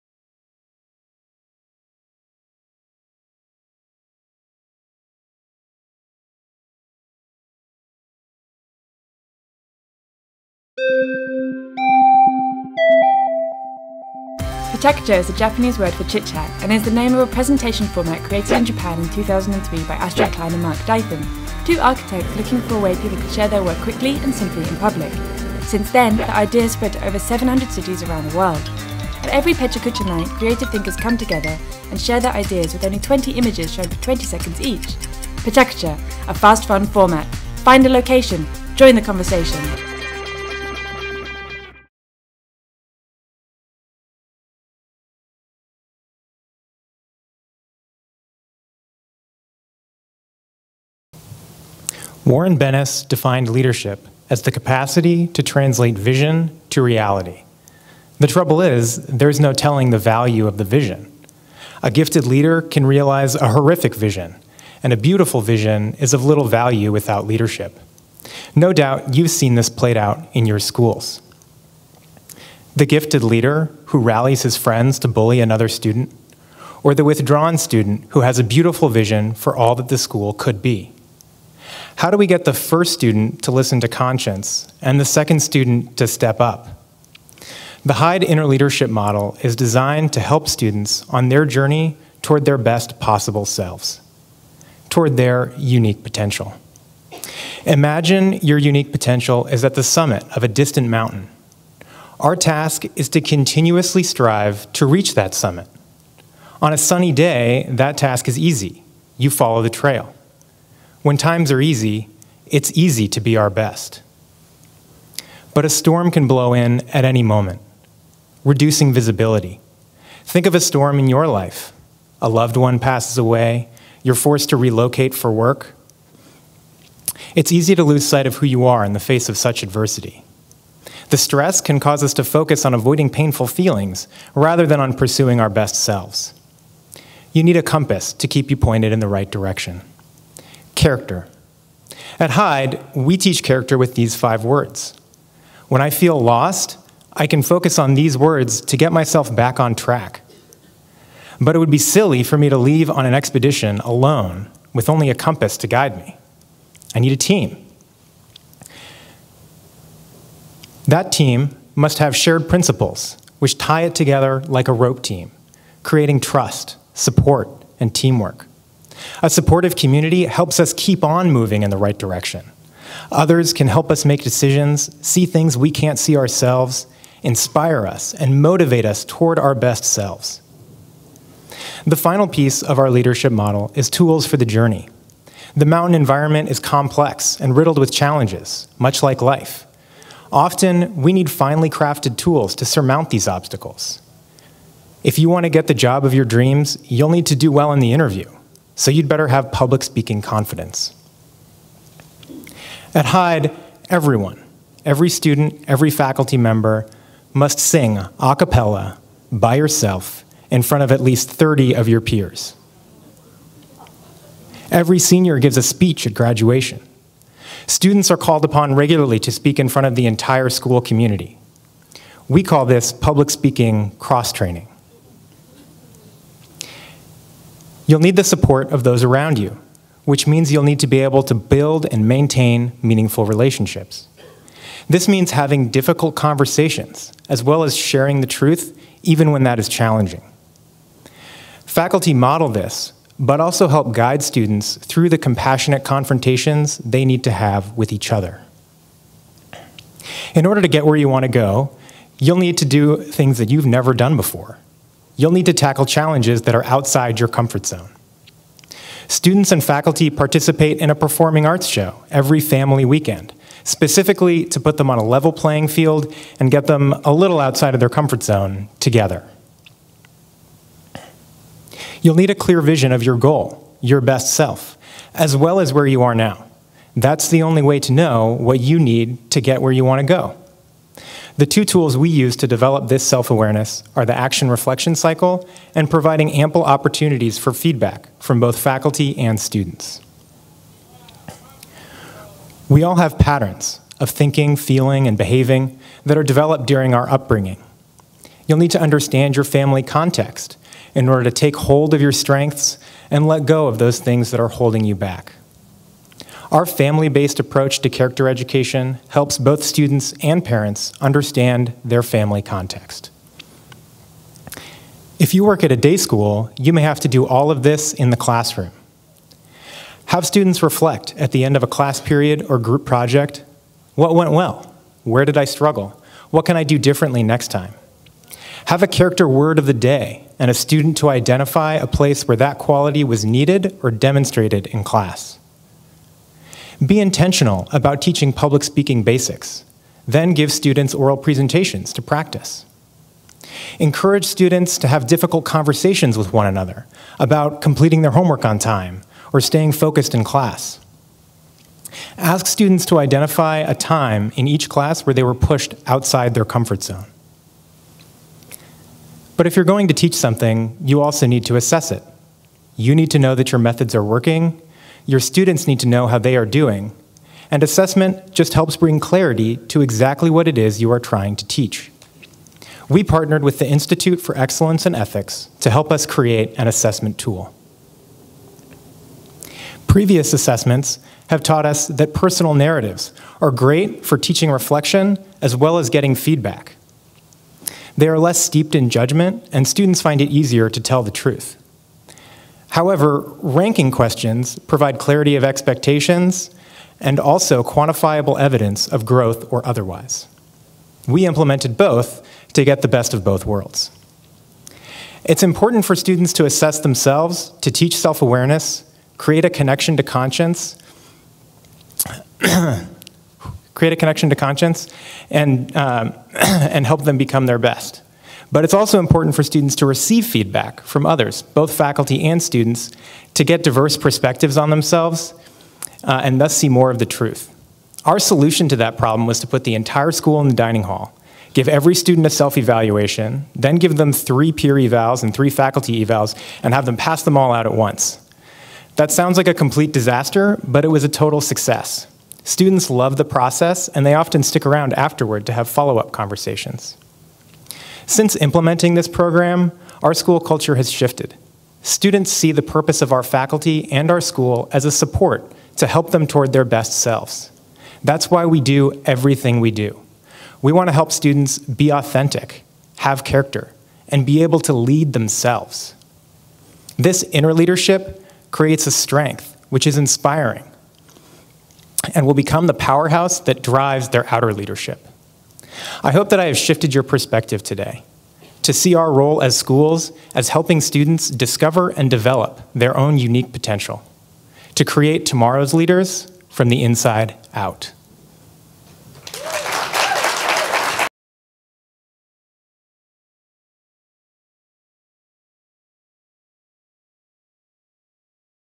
NAIS AC Powered by PechaKucha: Jacob Sargent
Jacob Sargent presents his PechaKucha, "Becoming Leaders in Our Own Lives from the Inside Out," at the 2018 NAIS Annual Conference.